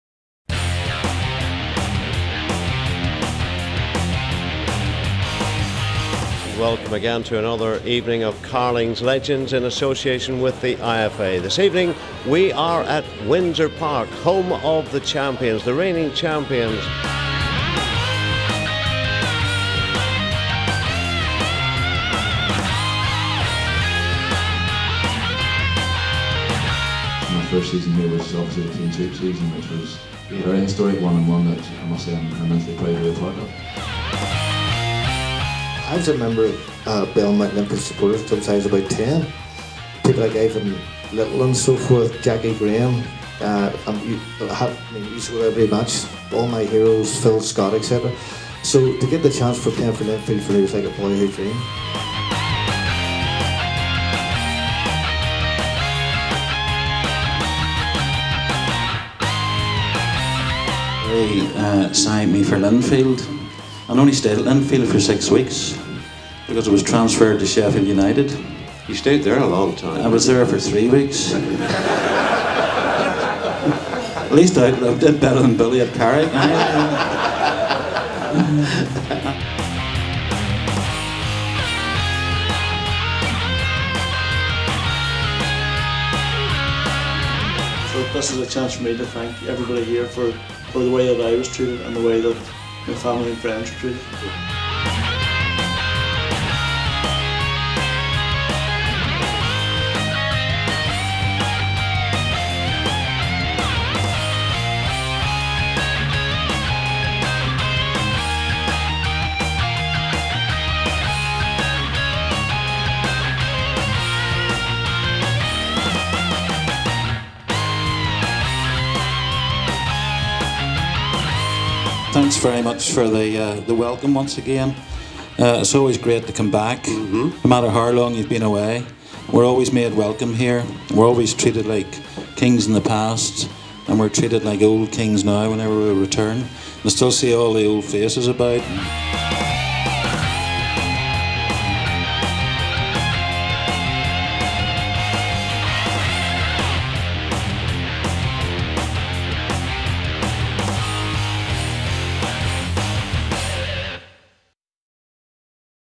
Linfield Carling Legends Night 2011
Carling Night of Legends hosted by Jackie Fullerton at Linfield Football Club. Music edit of night. Watch the full highlights online.